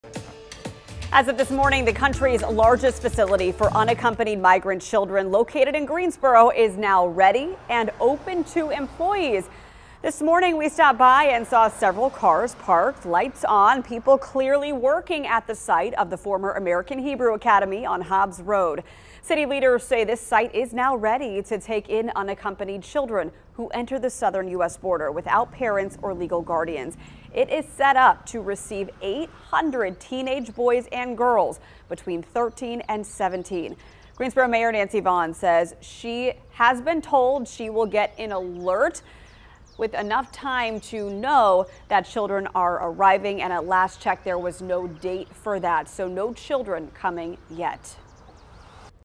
Greensboro migrant site officially open
City leaders say the site is now ready to take in unaccompanied children who entered the U.S. without parents or guardians.